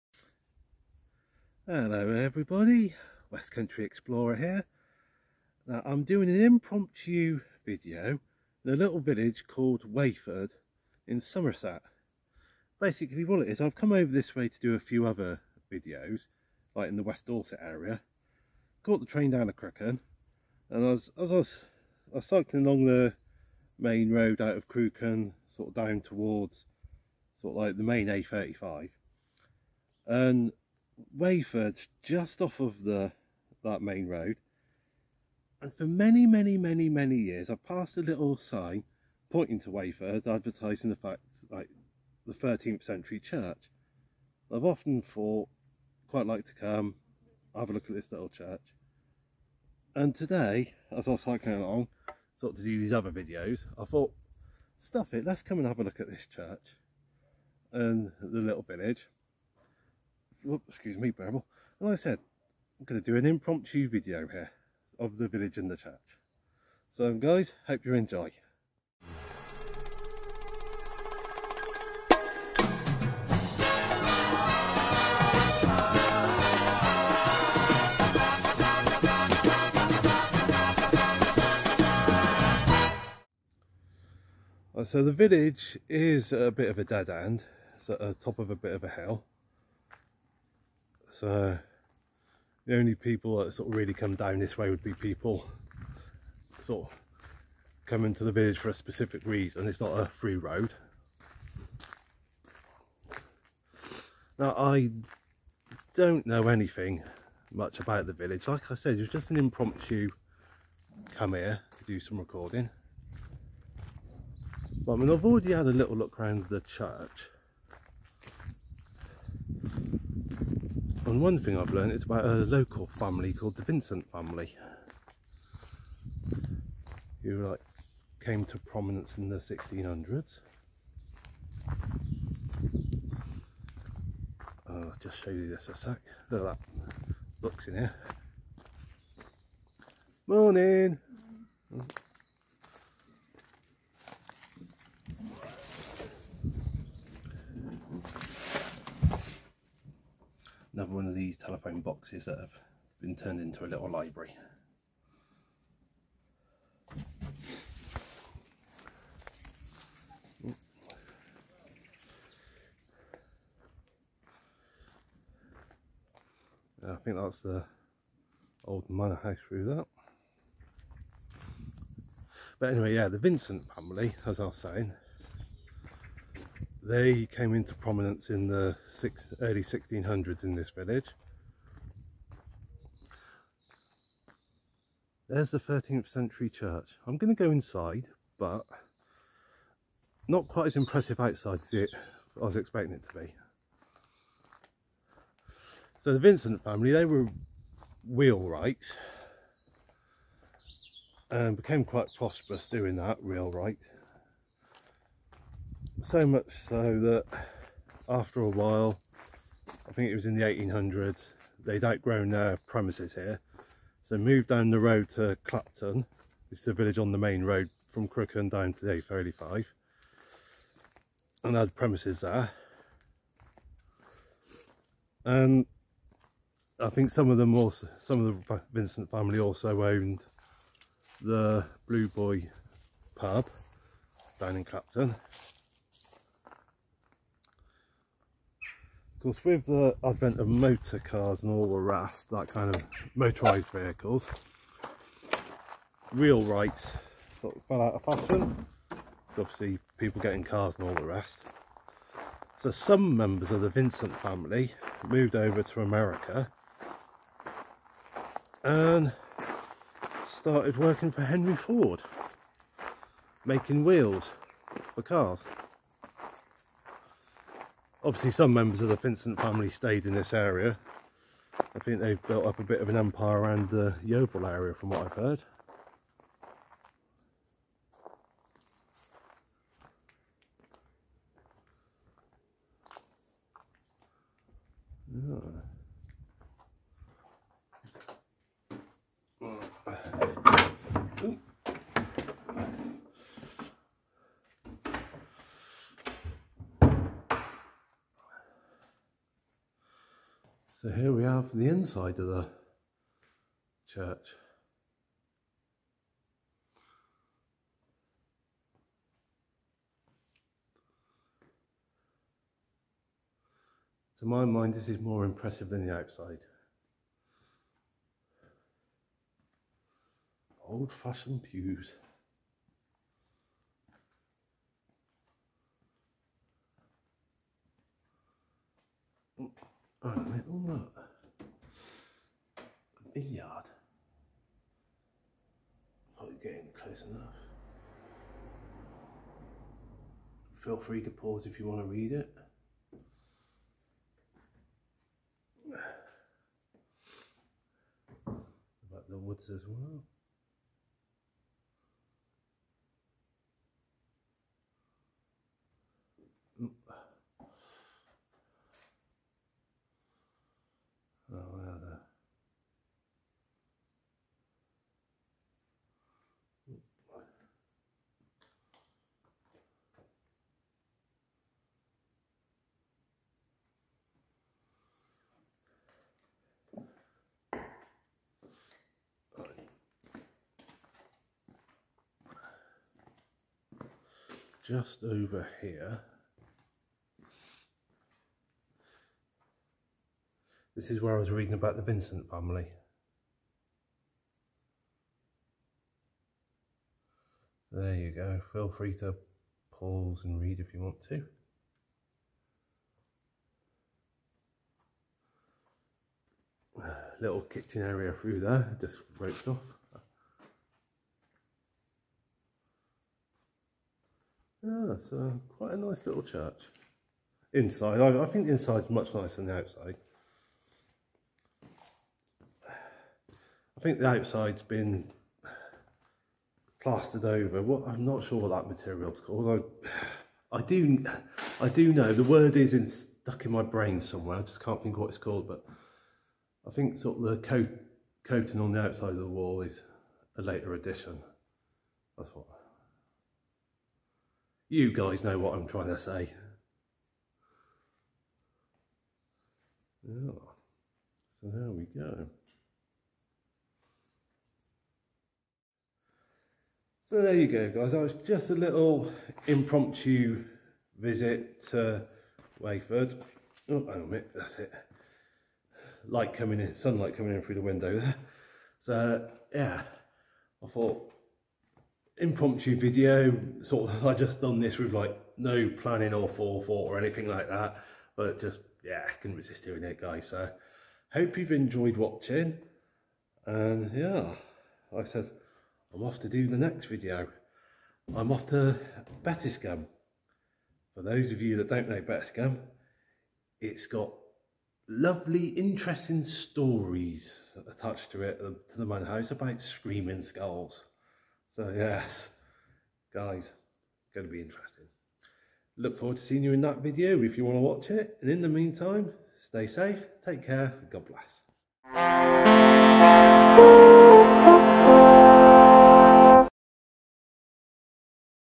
Warford, Somerset; An Impromptu Visit.
Whilst out & about on my bicycle today I made an impromptu decision to visit the little village of Wayford in Somerset.